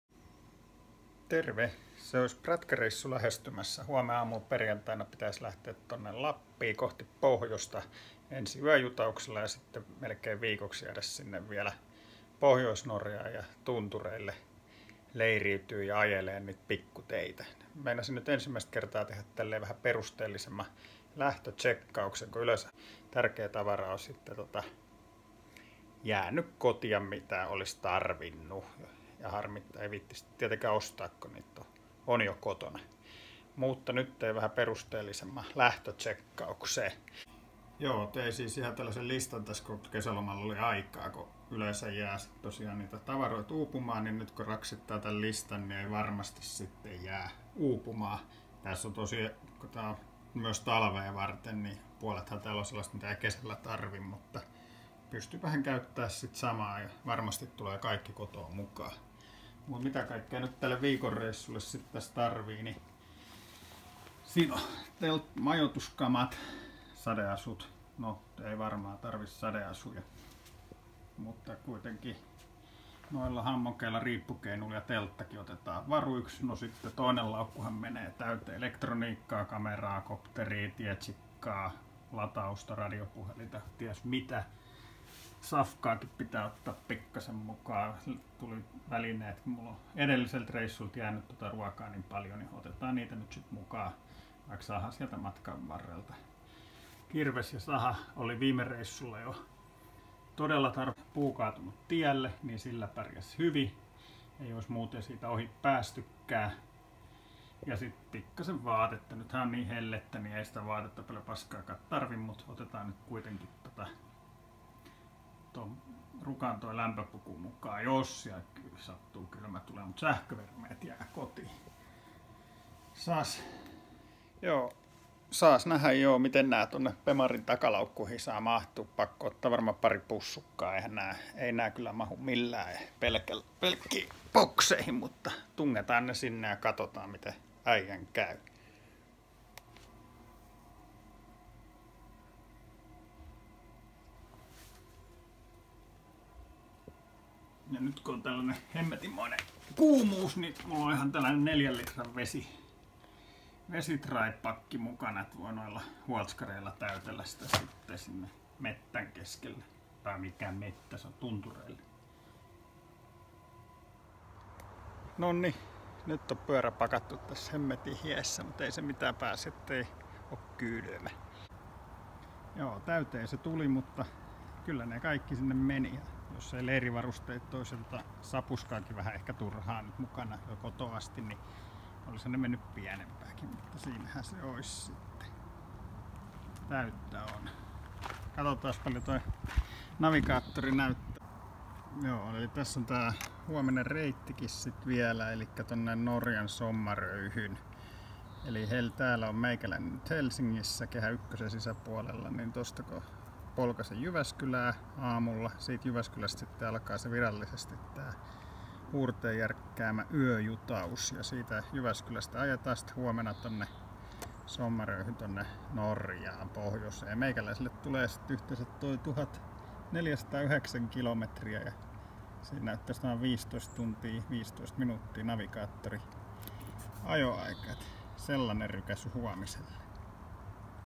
Gessun pakkaaminen
Viikon reissu pohjoiseen moottoripyörällä, mitä kaikkea mukaan, kun tarkoitus yöpyä myös